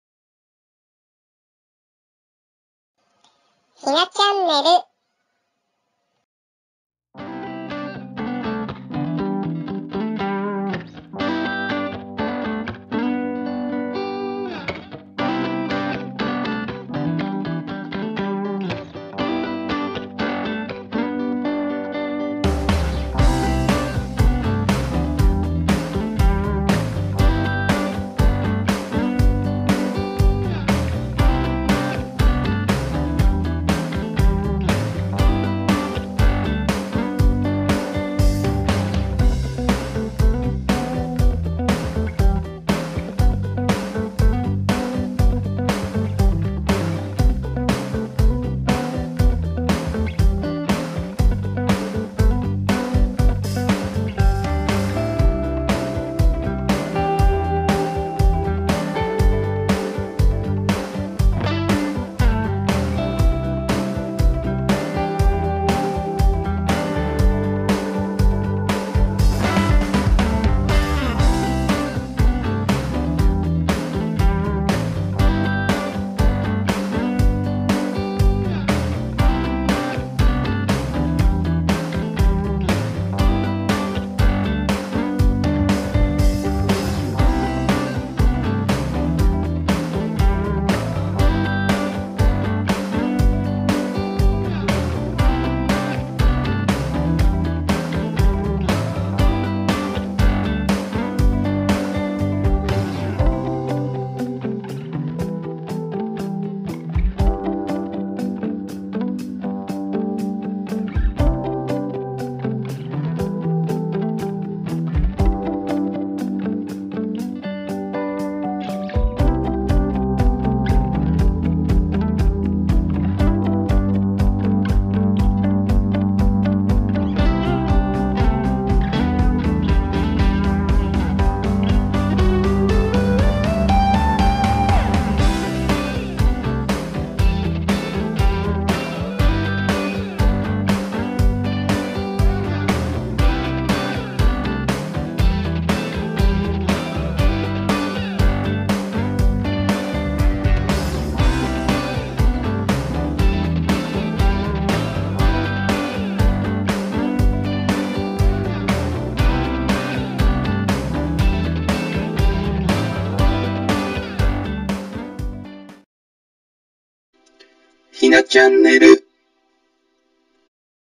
🐹ジャンガリアンハムスターがラップ芯のトンネルを走り抜ける hamster run through the tunnel
ハムスター(イエロージャンガリアン)がサランラップの芯のトンネルを走り抜けます。練習して臨んだかわいい様子をお楽しみ下さい。

寝る前の癒しのひとときになれば、より幸いです。

ひなちゃんねる

Hi!

This video is about the djungarian hamster.
Thank you for coming today!